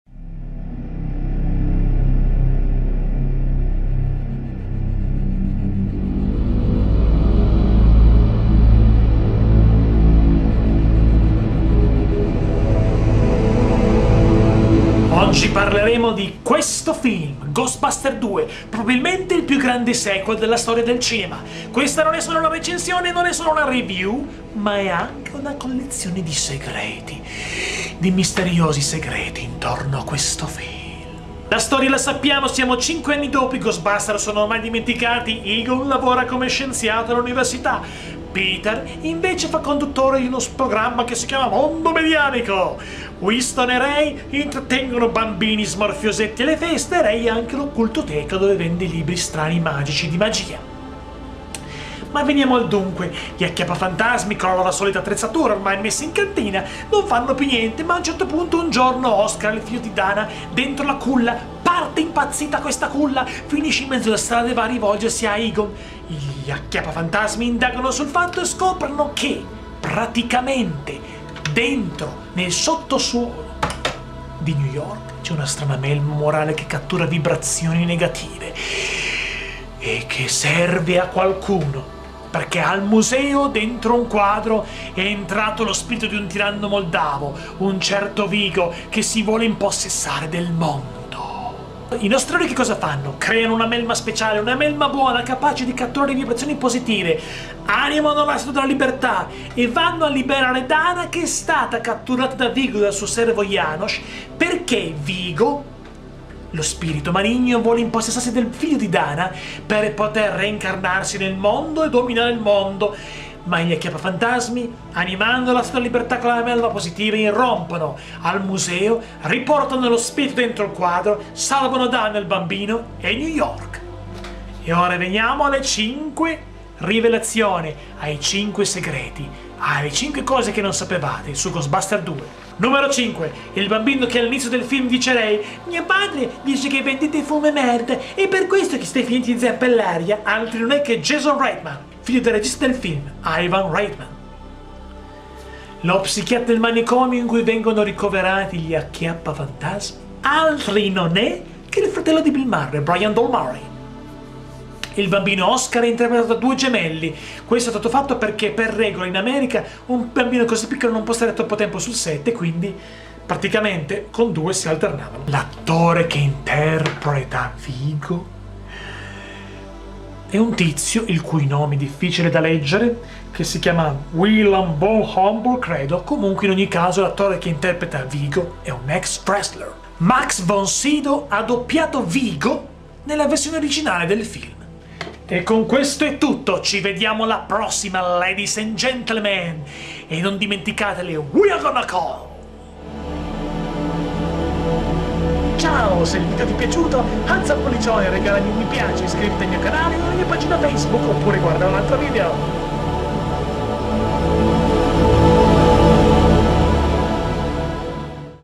Ghostbusters 2
Some Things That you Never Know on GHOSTBUSTERS 2
IN THIS VIDEO AND MORE. INCLUDING A special Review.